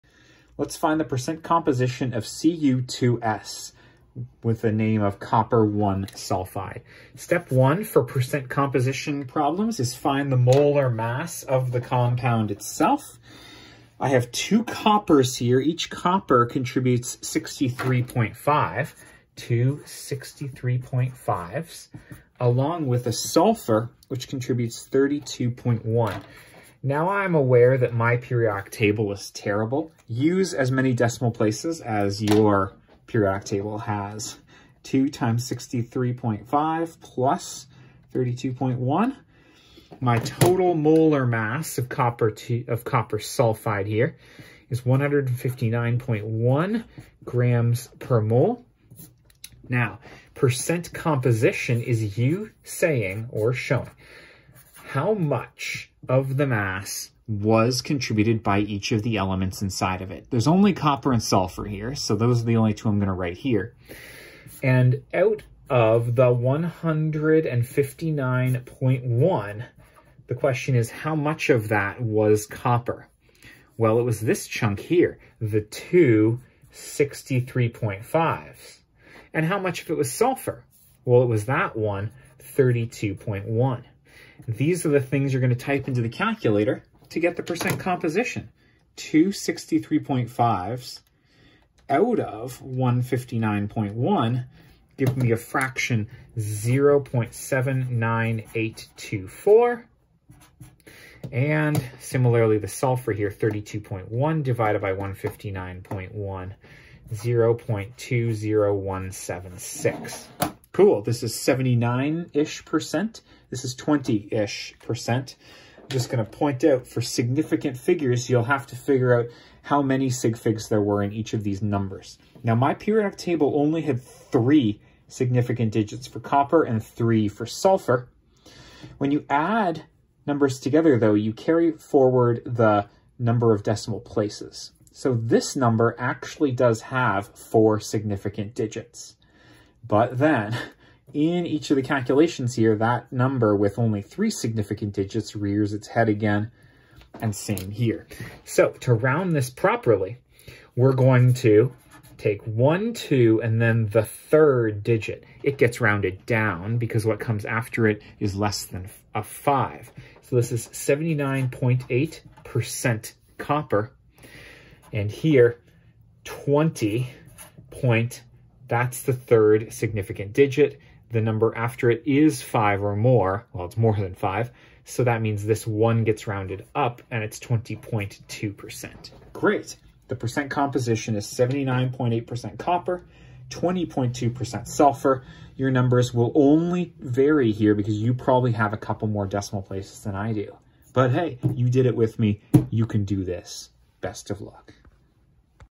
Find the Percent Composition of Cu2S, copper (I) sulfide
* Find molar mass of compound
* Divide each atom's contribution to the total molar mass BY the molar mass
* Convert to percentages